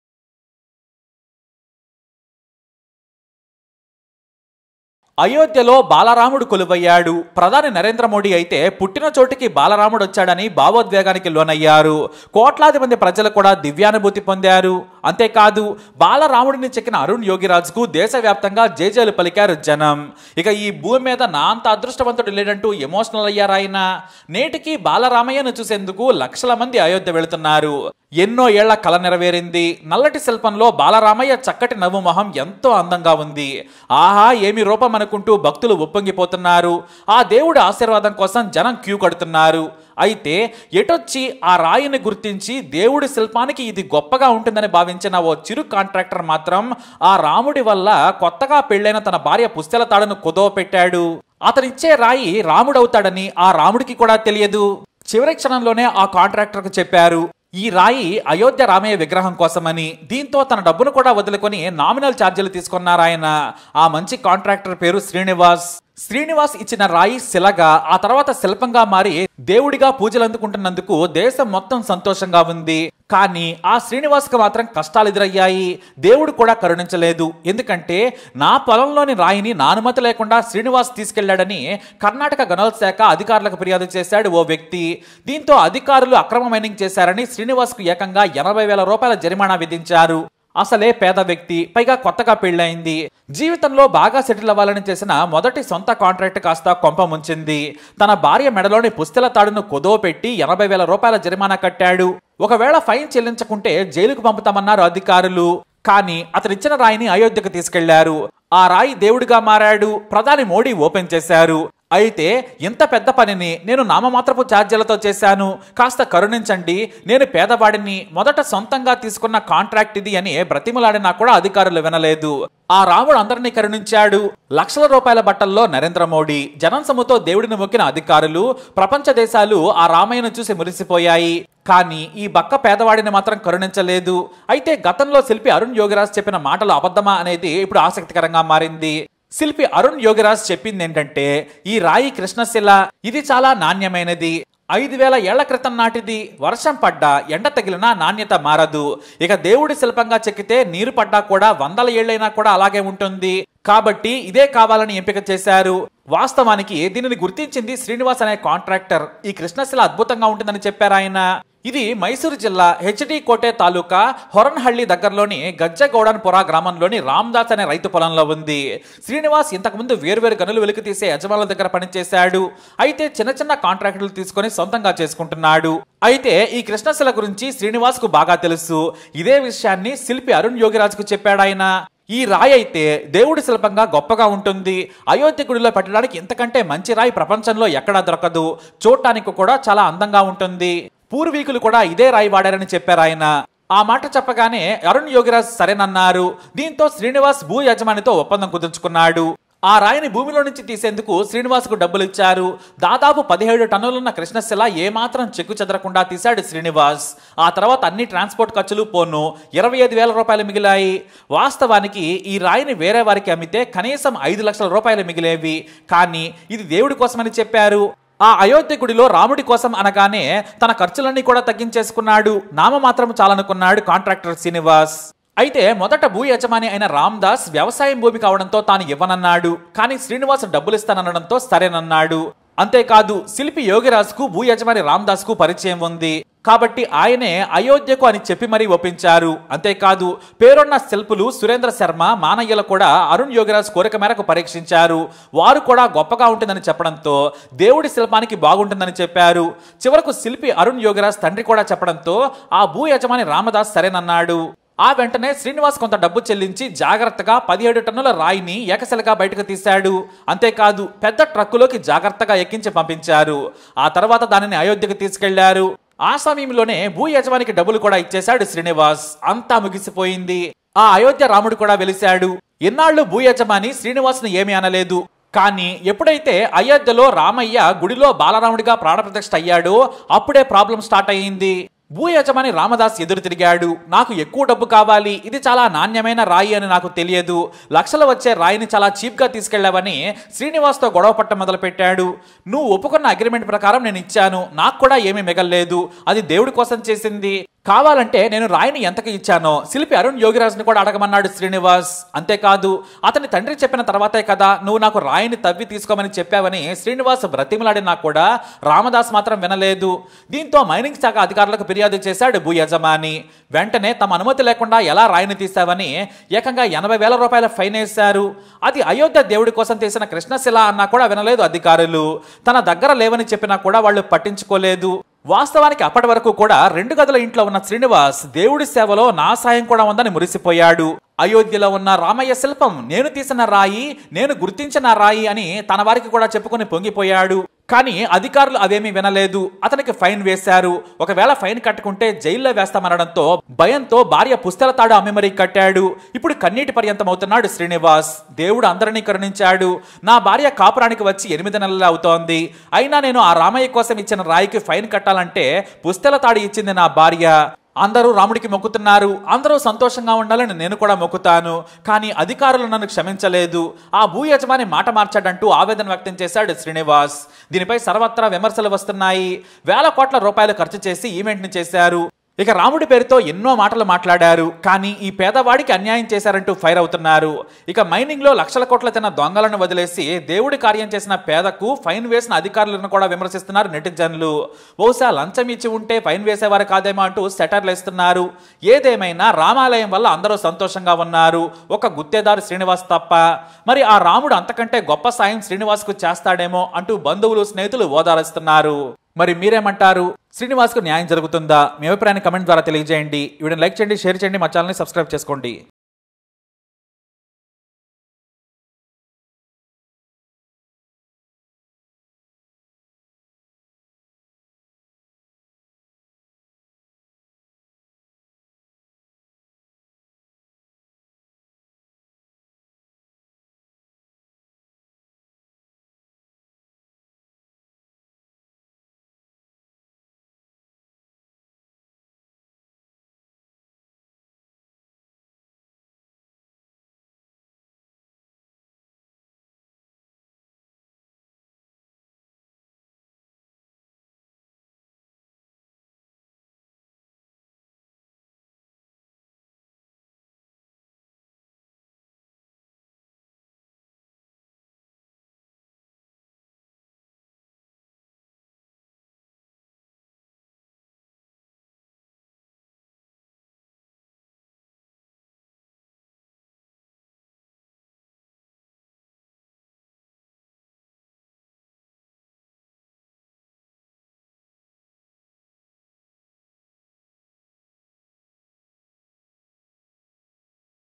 రాముడి కోసం రాయి ఇచ్చి జైలుకెళ్లాడు | Ayodhya Ram Idol Stone Contractor Srinivas Sad Story vov | SM
Watch ► రాముడి కోసం రాయి ఇచ్చి జైలుకెళ్లాడు | Ayodhya Ram Idol Stone Contractor Srinivas Sad Story vov | SM